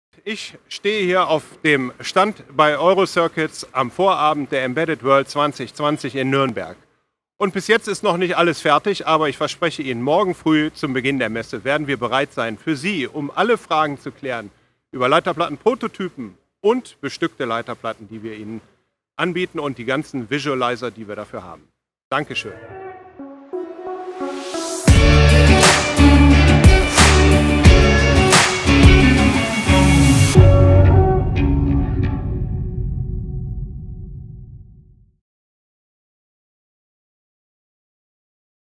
Willkommen zur Embedded World 2020
Das Team von Eurocircuits freut sich, Sie auf der embedded world 2020 in Nürnberg begrüßen zu dürfen. Sie finden uns in Halle 3A, Stand 135. Bis bald.